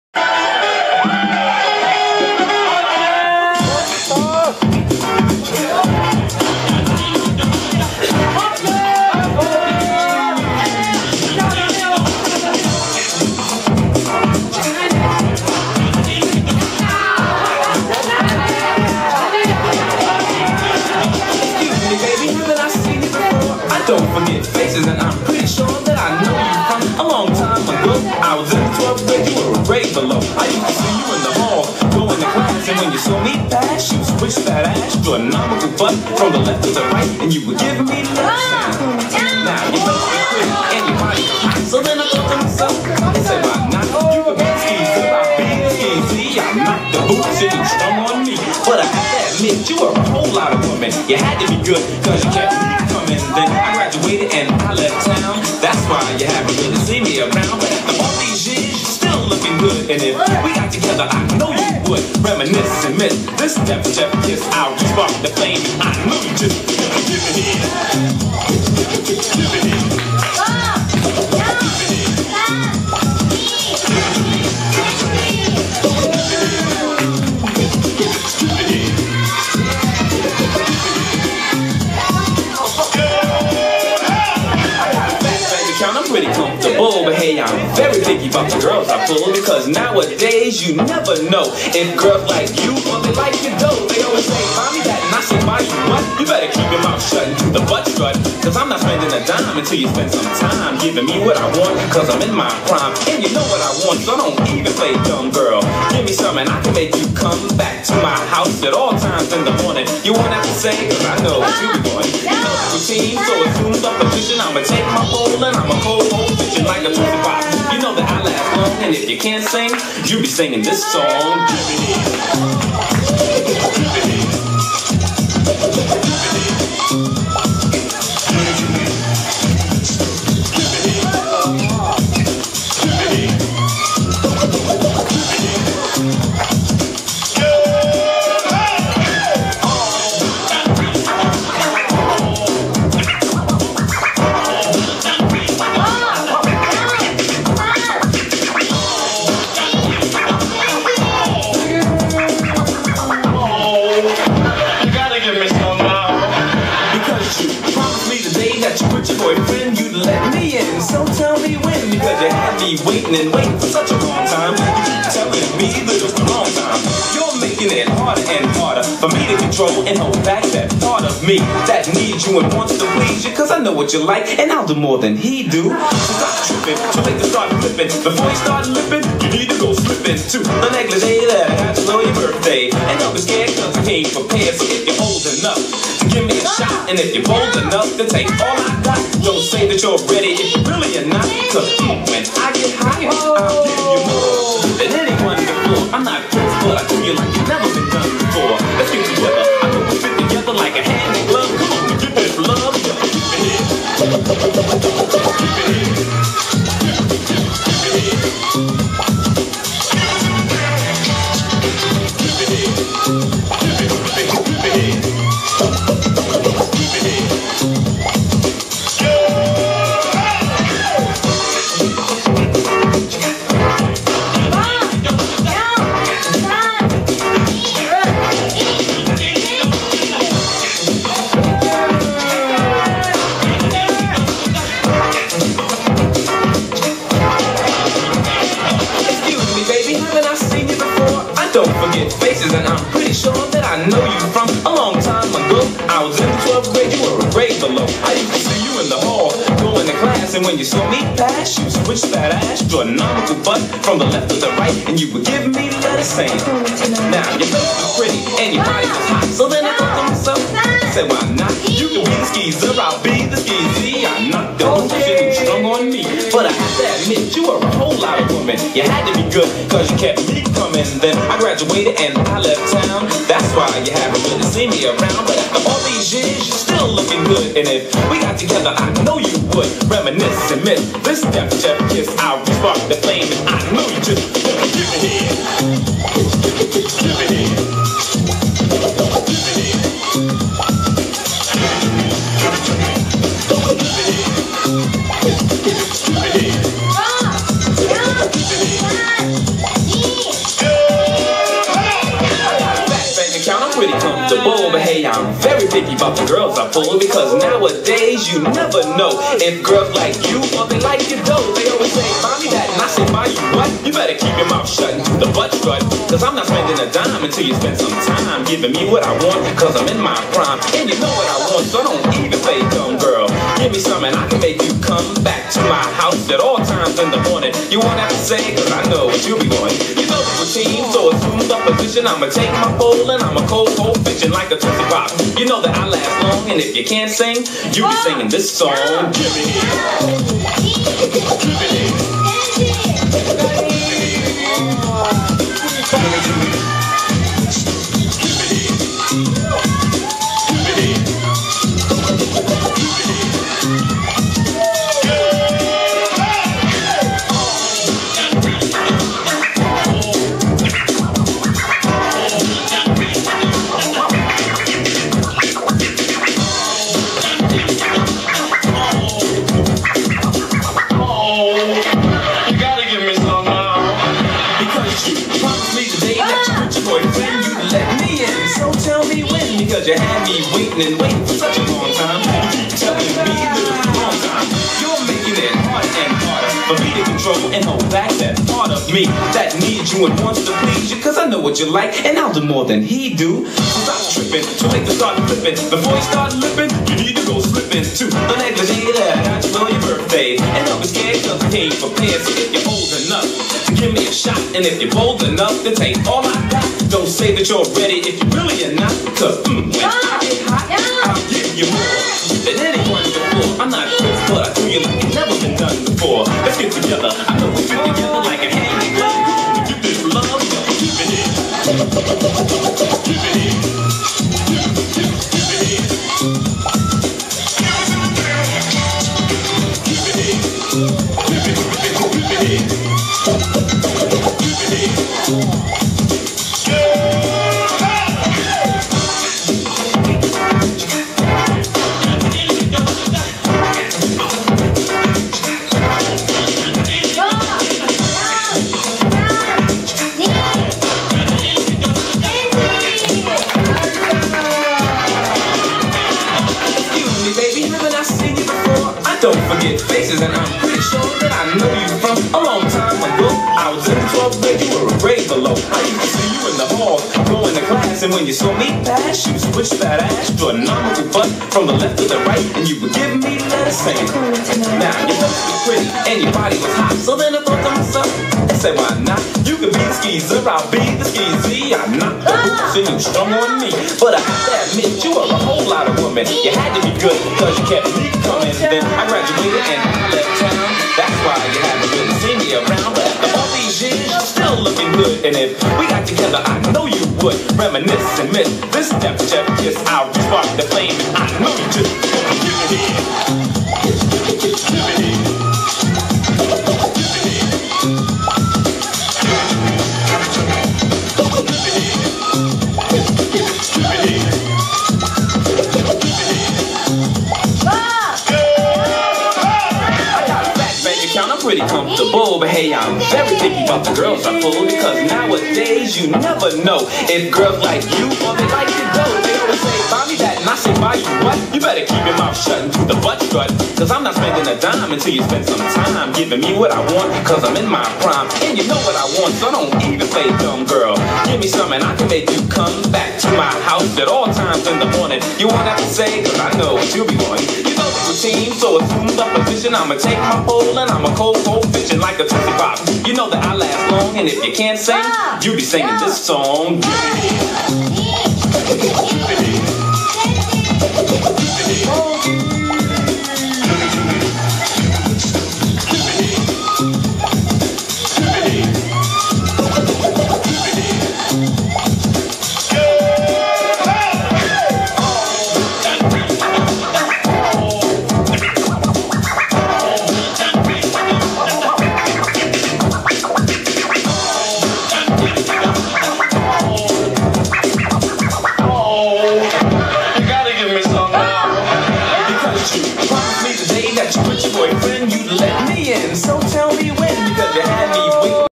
GROUND ZERO Vol.4 予選 Bサークル
2011.2.6
宇和島Boobyで行われたFREESTYLE SOLO BATTLE
「GROUND ZERO Vol.4」の予選になります！